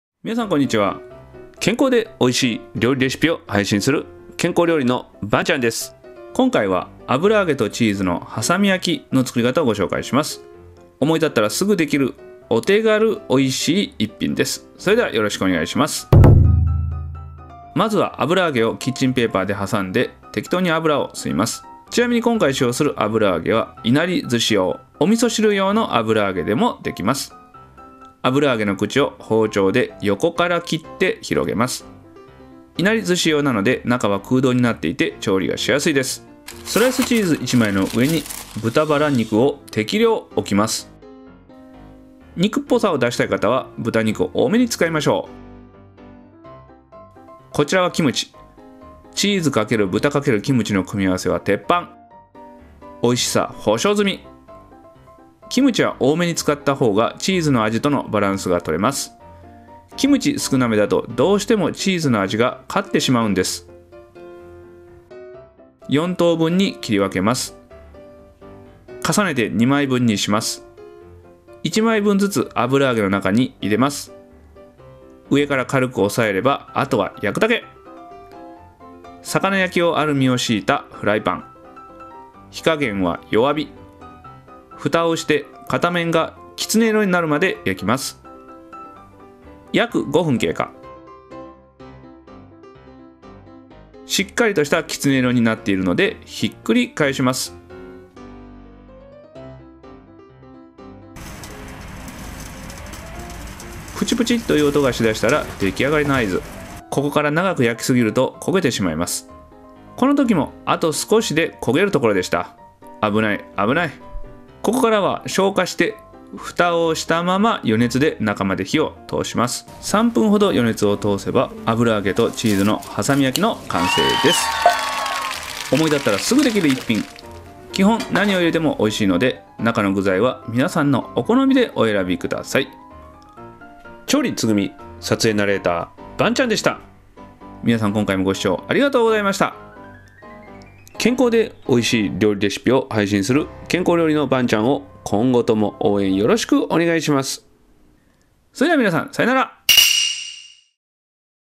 油揚げチーズ挟み焼きレシピ
【材料】①油揚げ②スライスチーズ③豚バラ肉④キムチ

調理：293
撮影ナレーター：ばんちゃん

※字幕2か所に誤りがあったので再編集してから再アップロードしました